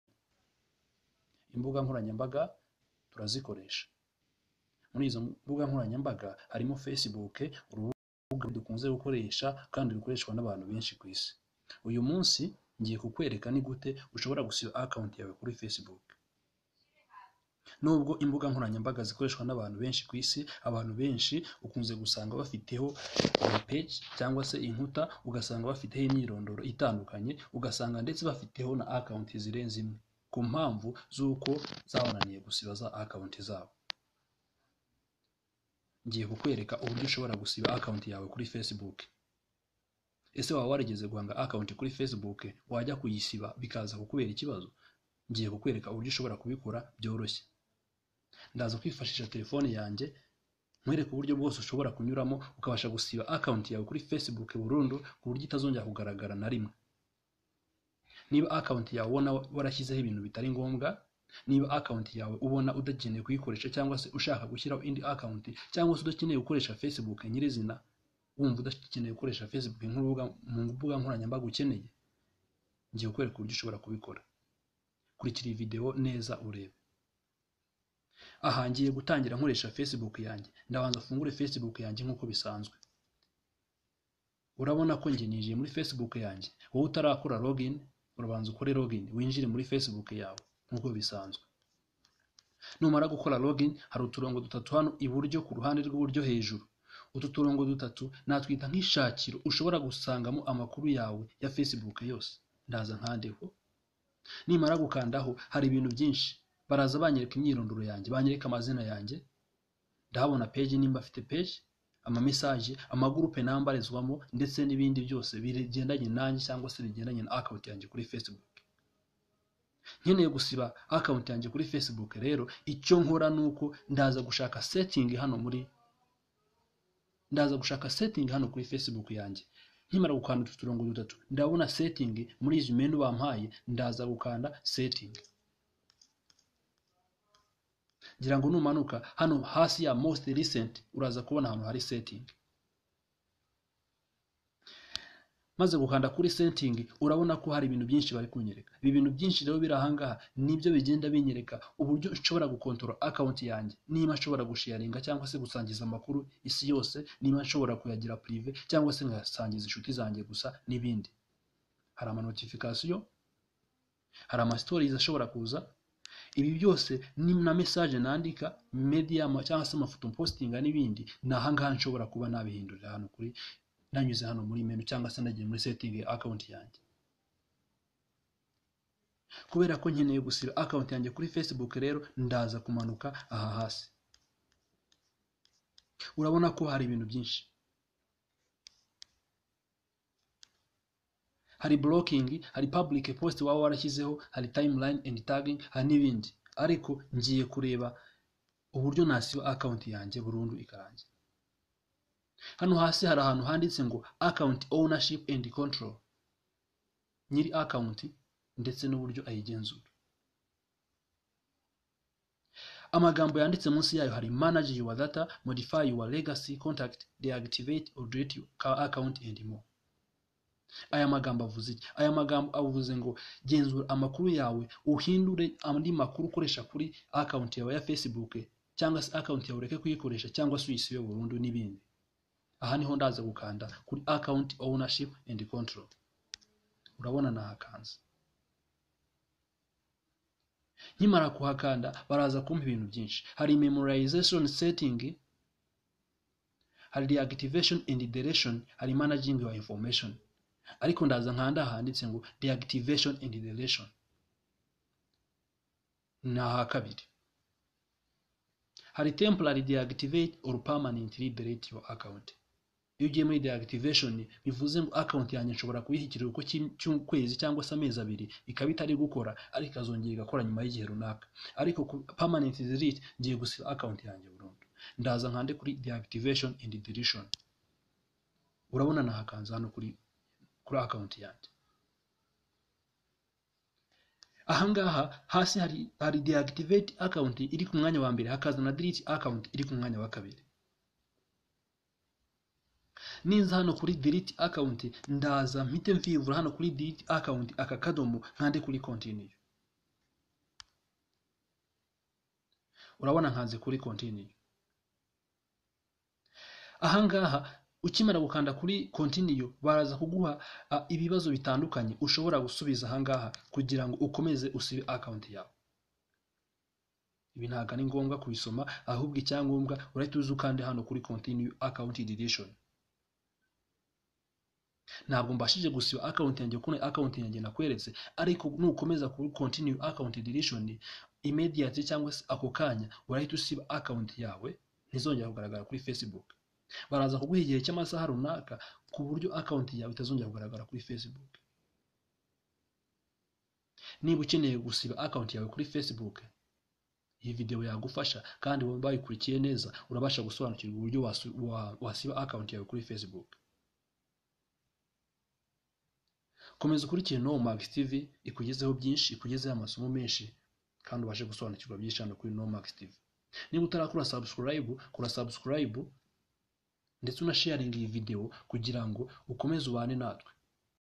Uko wasiba accountu yawe ya facebook burundu ntizongere kuboneka .
Ese wibagiwe password yawe kuri facebook none account yawe irashaka kuyusiba, Iyi video irakwereka uko wabikora. How to delete facebook account permanently.